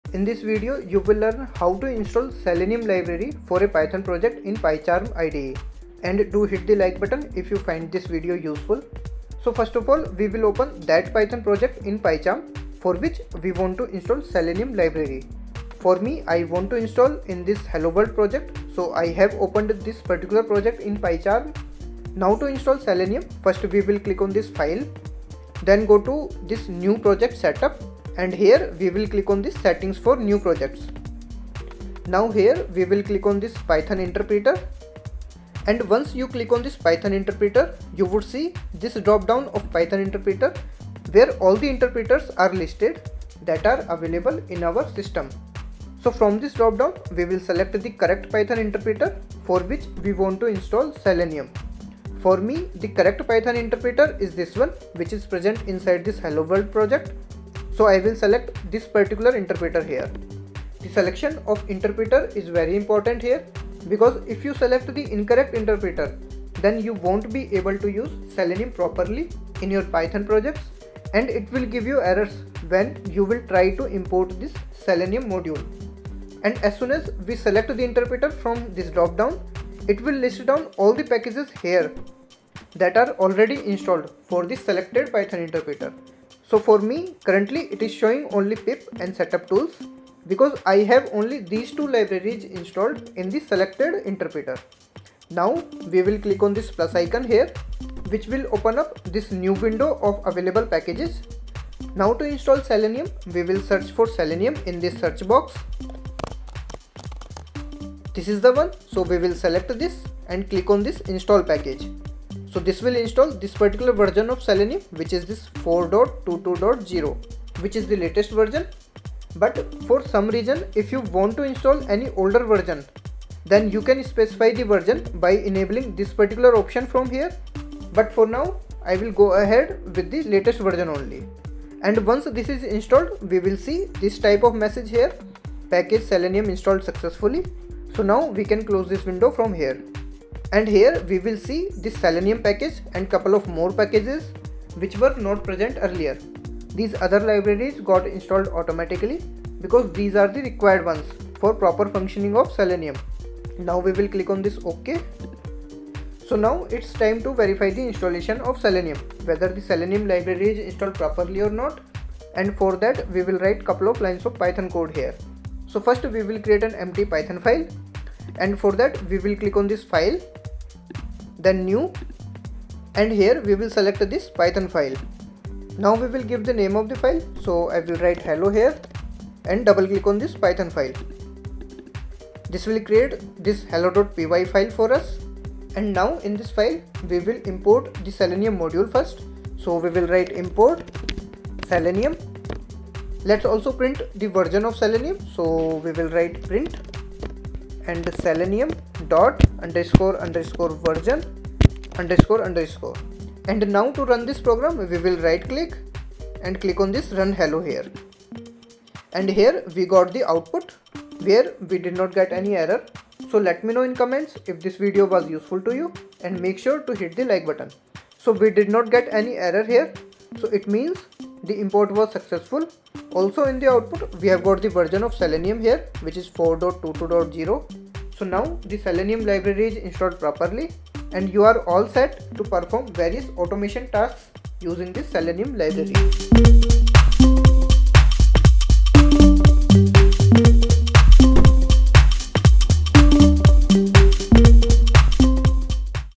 How to Install Selenium in Pycharm (Windows & Mac) [2024]
This video explains, how to install Selenium in Pycharm IDE. The Selenium Installation steps are valid for Windows and Mac OS.

In this video, I have explained the step by step process to easily install Selenium in the Pycharm IDE. And the mentioned steps are valid for both Windows (Windows 10, Windows 11) and Mac System (Macbook, Mac M1, Mac M2, Mac Pro, Air, Apple).

Learn how to install the Selenium library in PyCharm IDE for both Windows and Mac in this step-by-step tutorial! Whether you're a beginner or an experienced developer, this guide will help you get Selenium up and running quickly and easily.

📋 In this video, you will learn:
How to install Selenium in Pycharm
Setting up PyCharm IDE for Selenium development
Tips for troubleshooting common installation issues